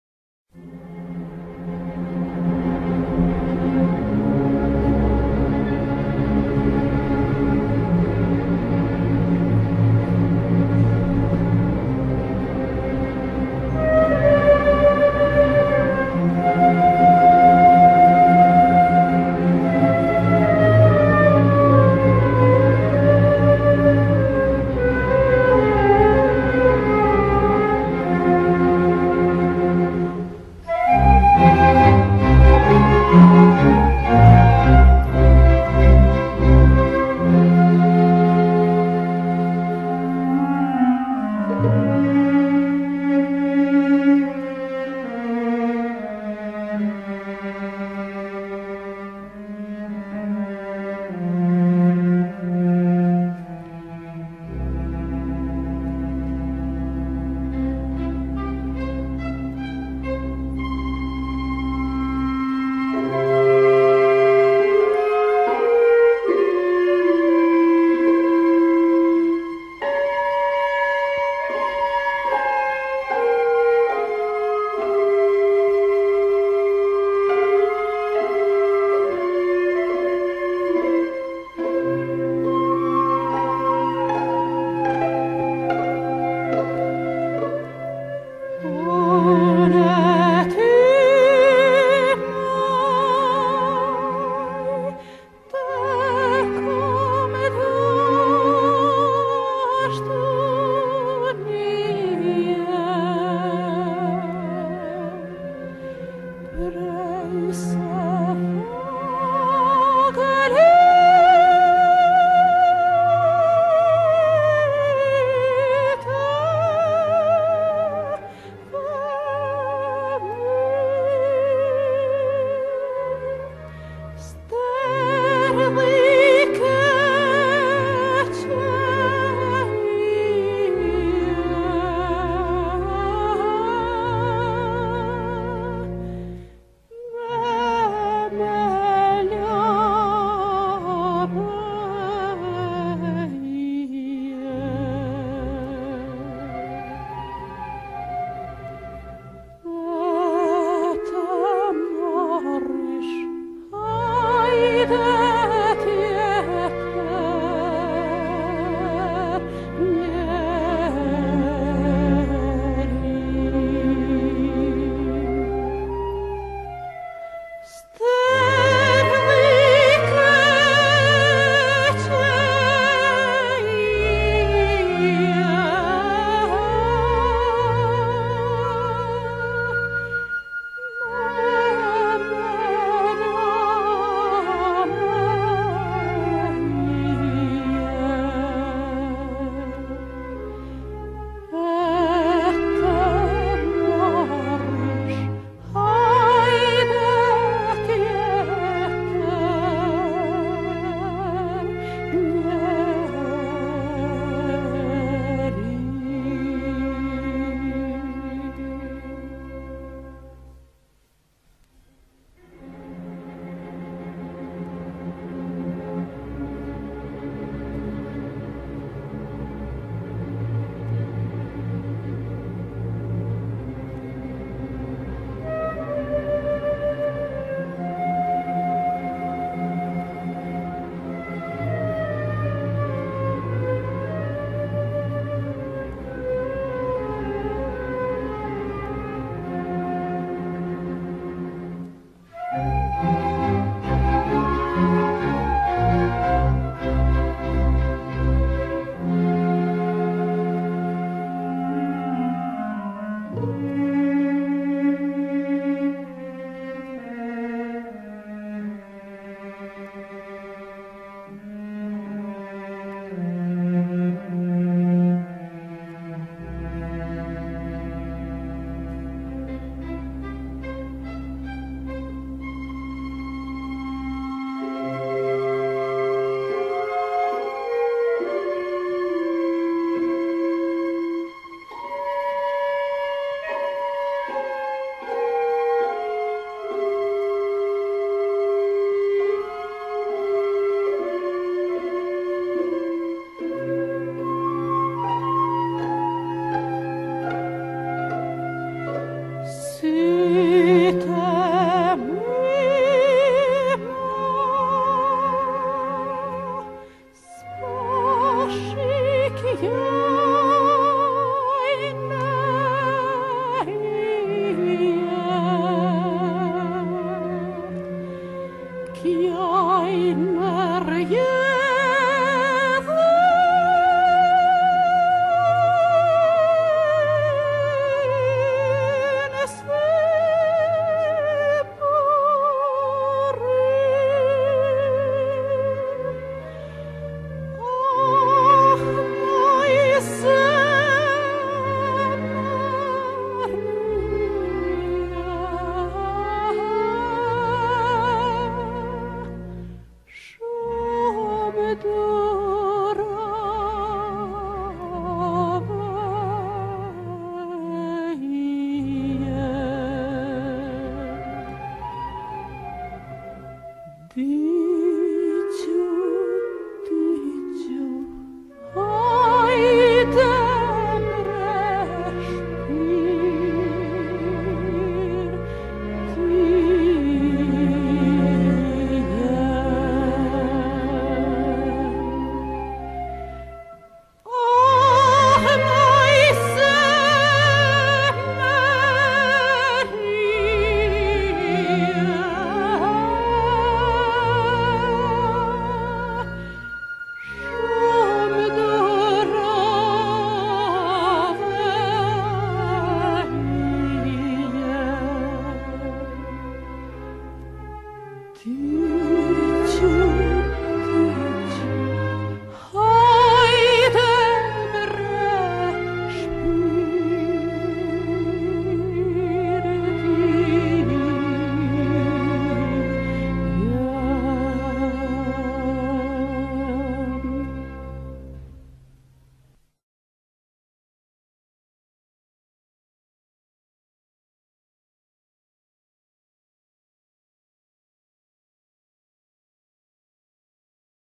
Foto të vjetra nga Elbasani (shoqëruar me këngë nga e famshmja Nexhmije Pagarusha)
Kjo video sjell për ju, pamje të vjetra nga qyteti i Elbasanit. Video shoqërohet nga një këngë e të famshmes Nexhmije Pagarusha.